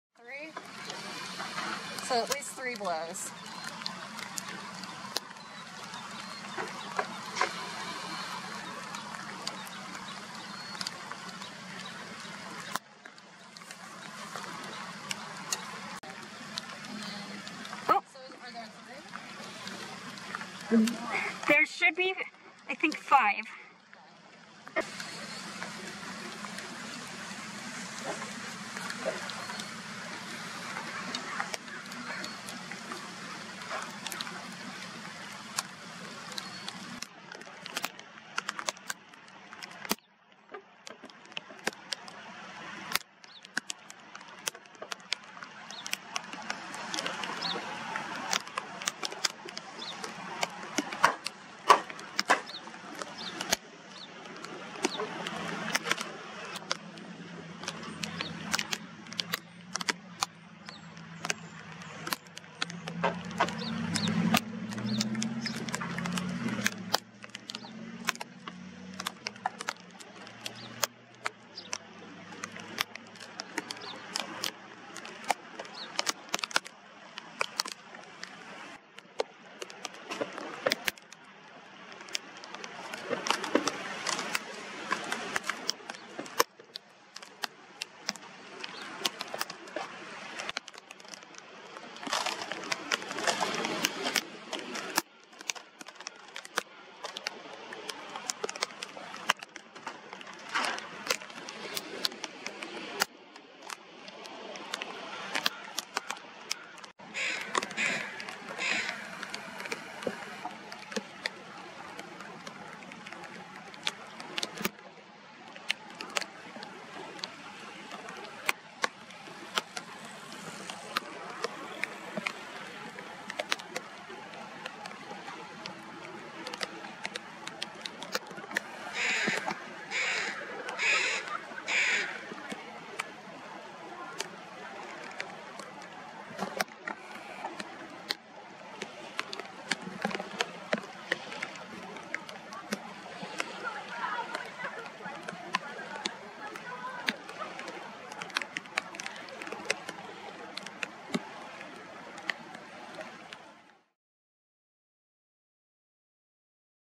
T123s West Seattle - April 18th, 2022
Footage taken in West Seattle, at Beach Dr. SW and Lincoln Park on Monday, April 18th, 2022. The matrilines present was the T123s, who are members of the Transient/Bigg's (mammal-eating) killer whale population. Prior to our arrival, T124C had been traveling with the T123s, but had departed and headed in a separate direction.

The T123s are made up of the mother T123 “Sidney” (36-year-old female), T123A “Stanley” (21-year-old male), T123C “Lucky” (9-year-old female), and T123D “Darcy” (3-year-old female).